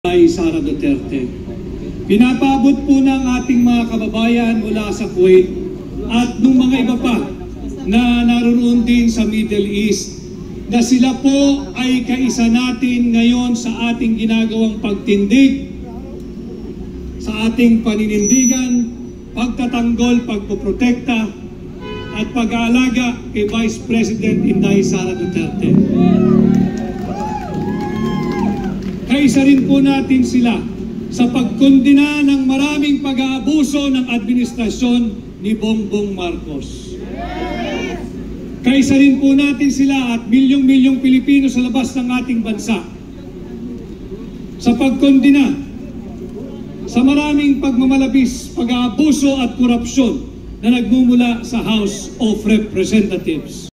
Atty. Vic Rodriguez may banat | VP Sara ipinagtanggol
Muling binanatan ni Atty Vic Rodriguez ang mga mambabatas kung saan pilit na ginigipit ang OVP at si VP Sara Duterte